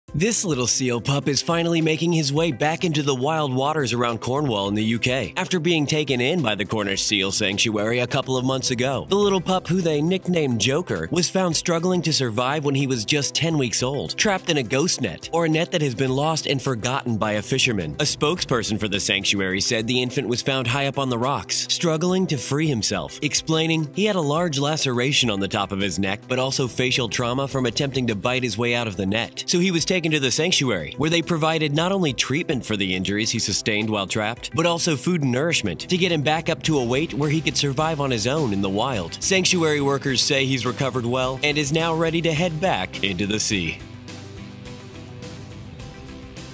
Rescued Seal Released After Being Caught in a Ghost Net
The Cornish Seal Sanctuary says goodbye to one of their recently rehabilitated baby seals. Buzz60’s Tony Spitz has the details.